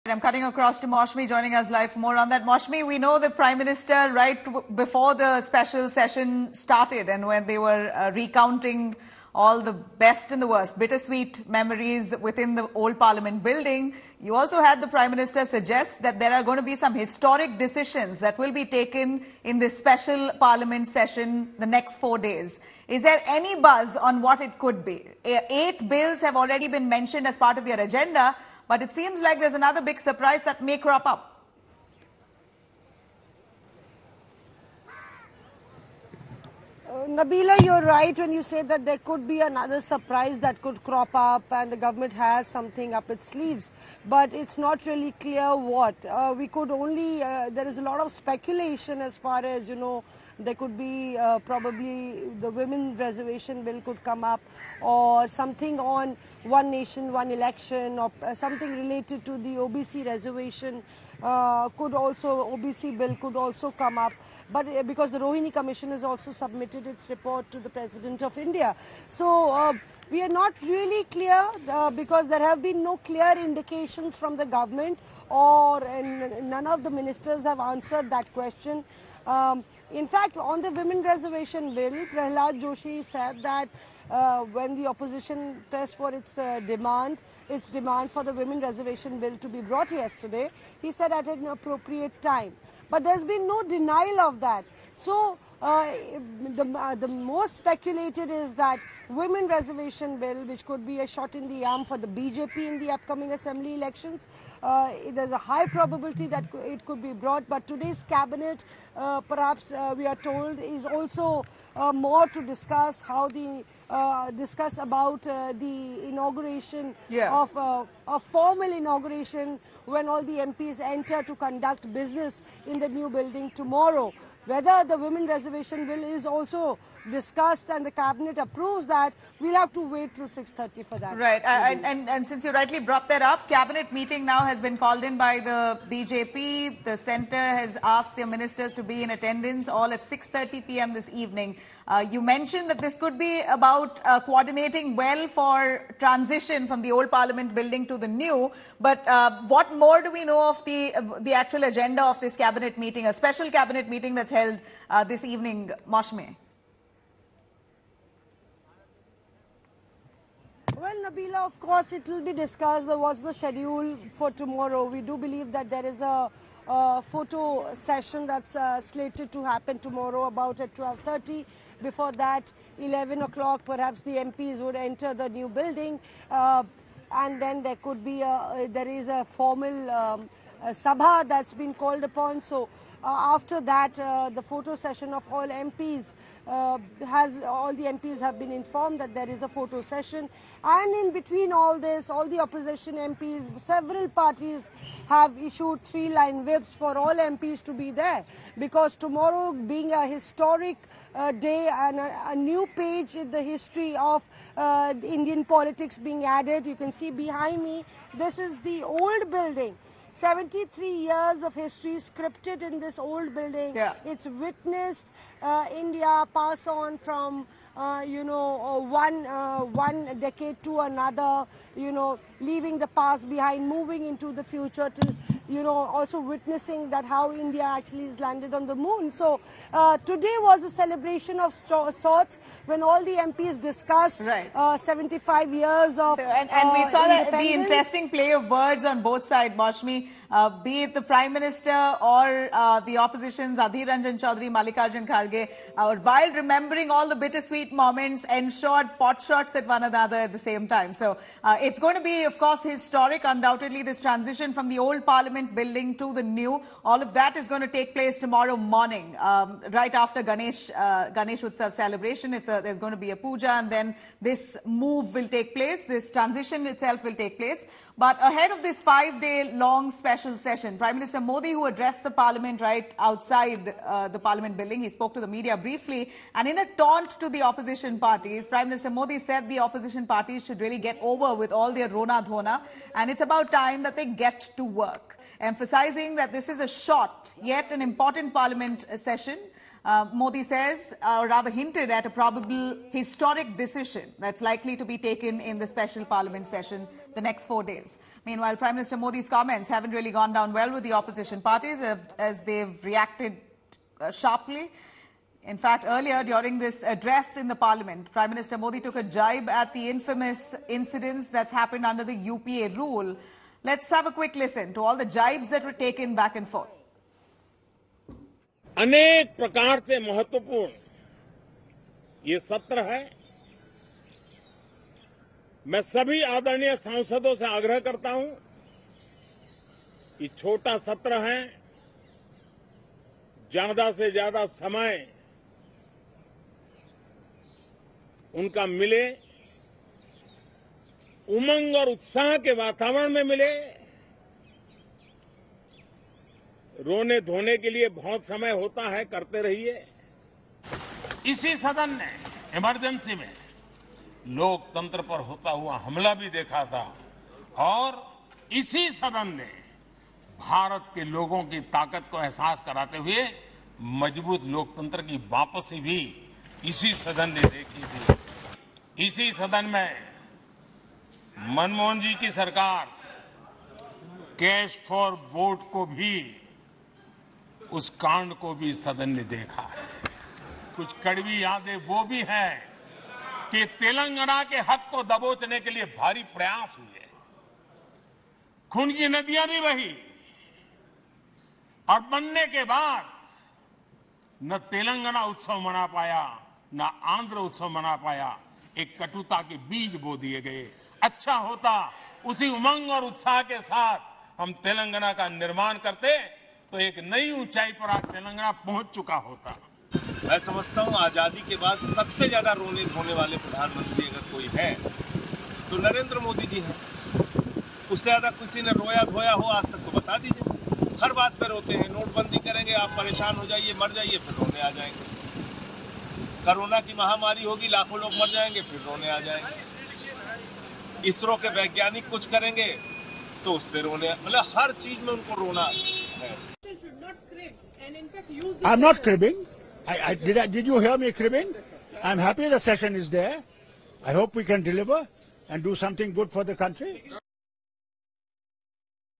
Cabinet Meeting Likely To Discuss New Bills, Opposition Demands Fair Quota For Women
The five-day-long special session of Parliament begins today amid intense speculation about whether the government will make any surprise moves. The government has listed eight bills to consider during this session, including a bill to bring changes in the selection of Election Commissioners. On Monday, the session will see a discussion on the 'Parliamentary Journey' spanning 75 years, and it will be opened by Prime Minister Narendra Modi in the Lok Sabha and Union Minister Piyush Goyal in the Rajya Sabha. Additionally, the session will witness the relocation of Parliament proceedings to the new building